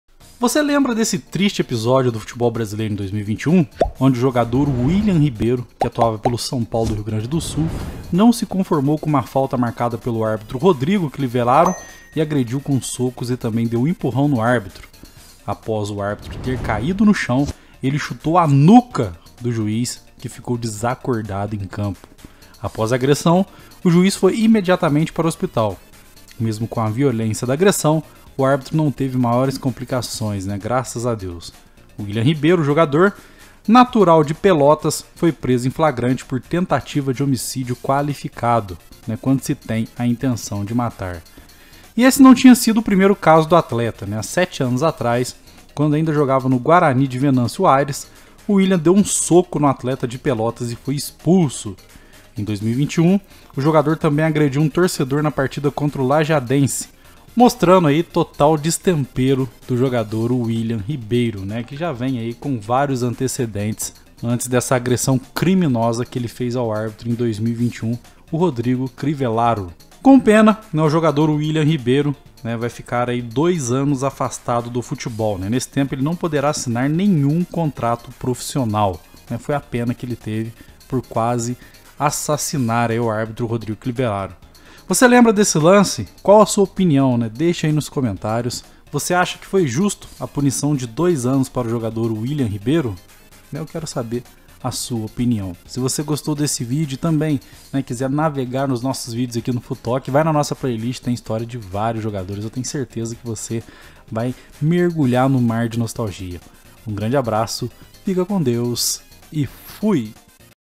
O QUE ACONTECEU COM WILLIAM RIBEIRO - JOGADOR QUE CHUTOU A CABEÇA DO ARBITRO RODRIGO CRIVELLARO
Voce lembra dessa cena lamentável do futebol brasileiro ? Lance que o jogador William Ribeiro agrediu um arbitro no Rio Grande do Sul . Deixe sua opinião !!!